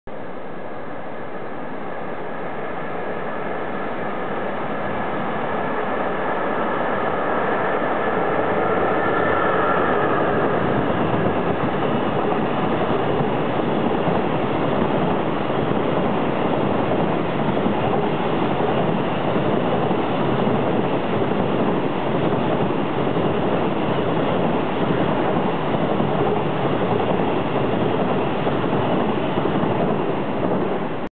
JR山陽本線兵庫駅 貨物列車通過 Freight train passing Hyogo station
EF66形100番台電気機関車が牽引するコンテナ列車。和田岬線のホームより撮影。
Tren de carga que pasa de Hyogo estación.Un train de marchandises passant gare Hyogo.Грузовой поезд проходящий Хиого станции.Angkutan kereta api yang lewat stasiun Hyogo.